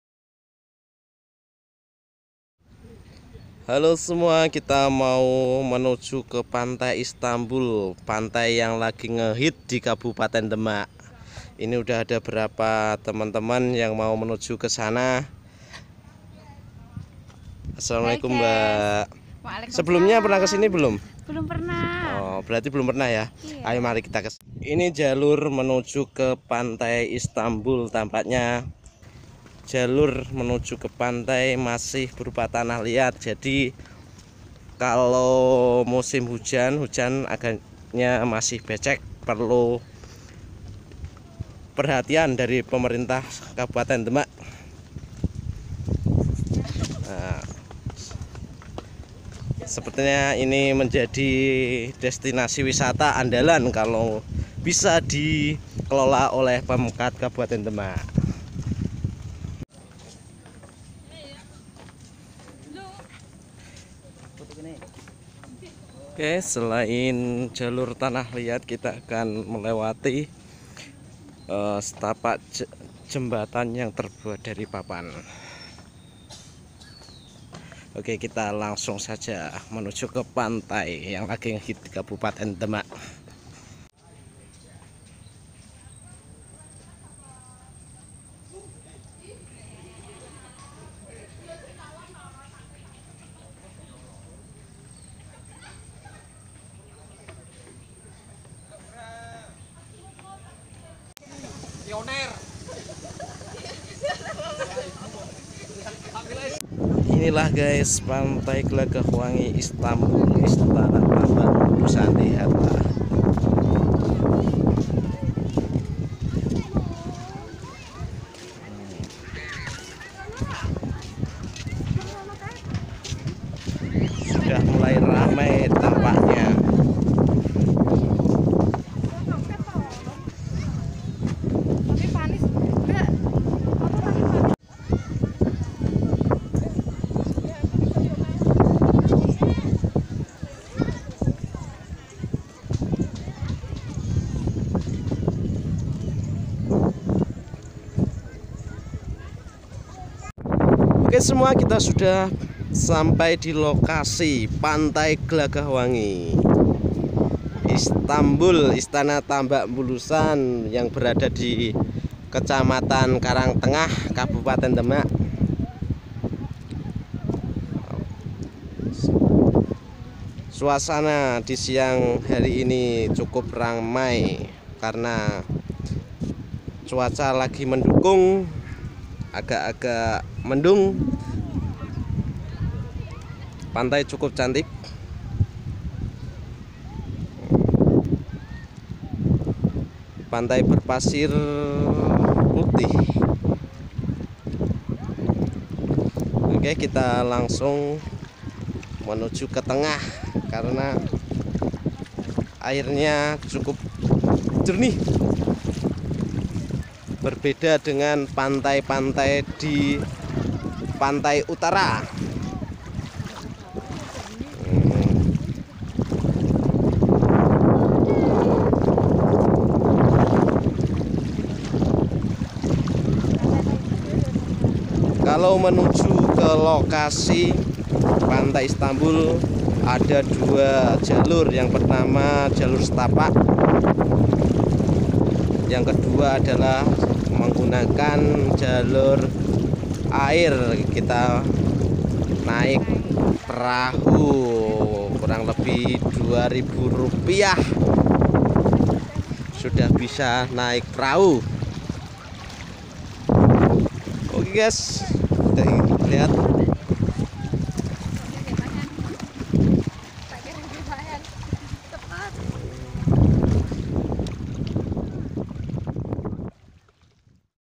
Penemuan pantai cantik di kab. Demak, Pantai Glagah Wangi Istambul
Pantai Glagah Wangi Istambul ( Istana Tambak Bulusan ) merupakan pantai yg terletak di desa tambak bulusan kec. Karang tengah kab. Demak..